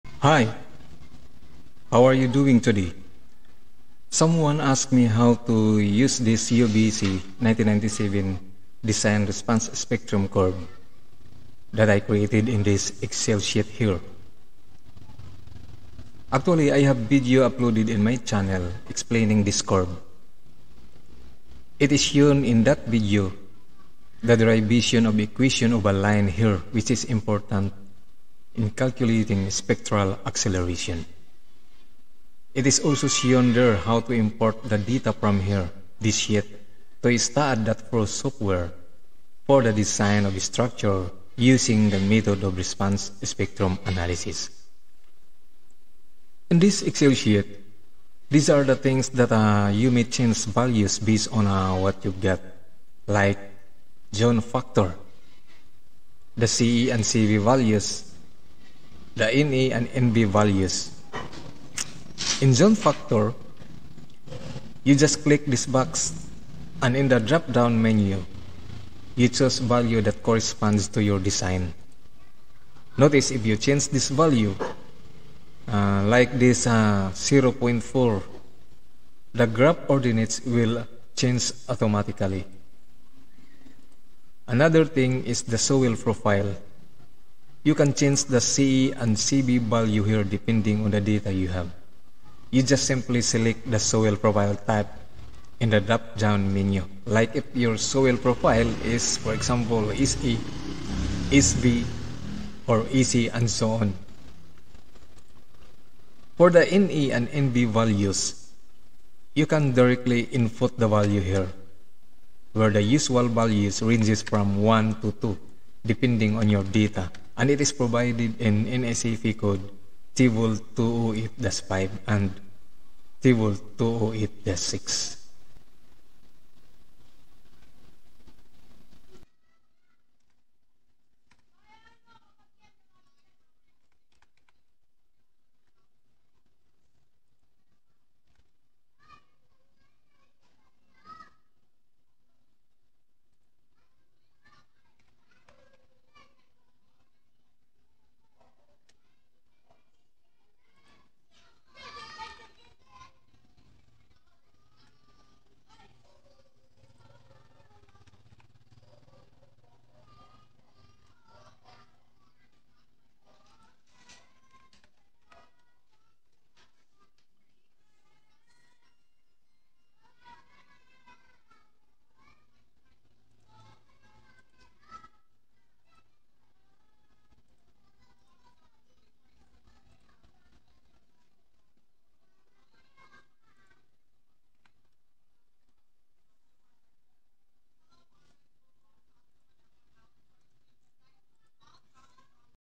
AUTOMATE UBC 1997 RESPONSE SPECTRUM CURVE...
In this video, we will learn how to set up the date of specific Site Seismic response spectrum data in the spreadsheet to automate the changes, everytime  different  soil profile type is used in the design.